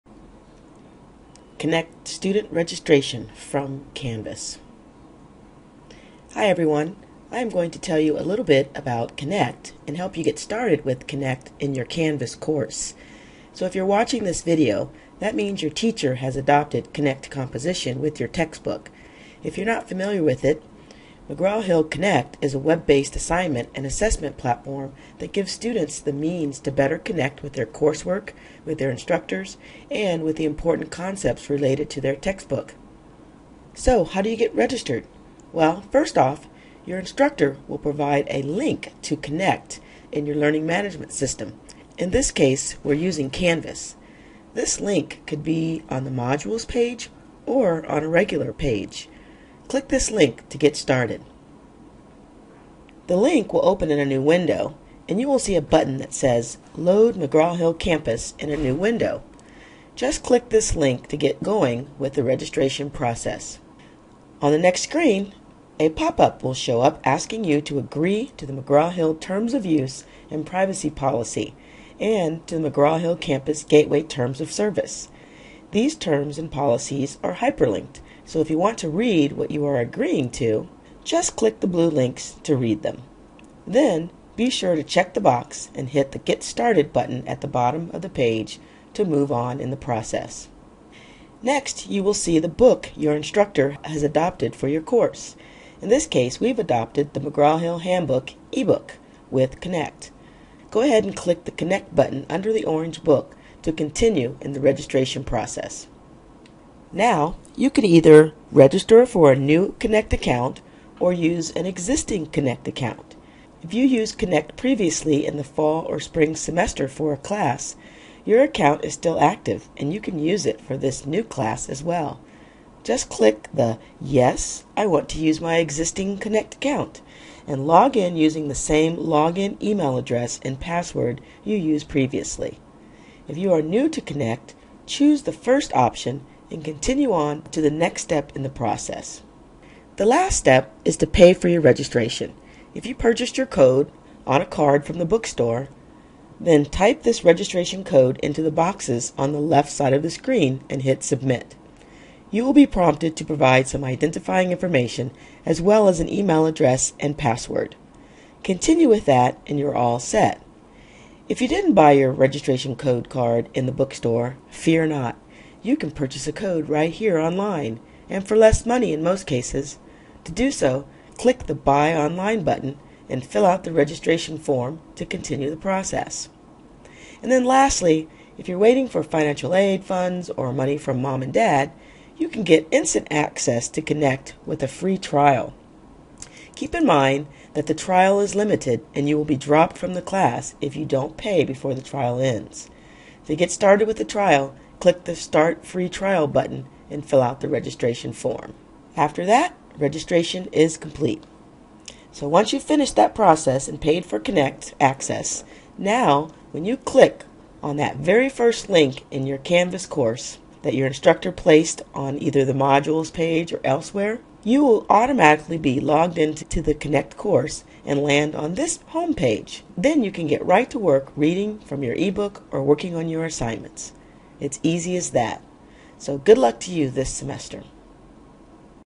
Connect Student Registration from Canvas
Video to help students get registered for Connect Composition from within an Instructure Canvas course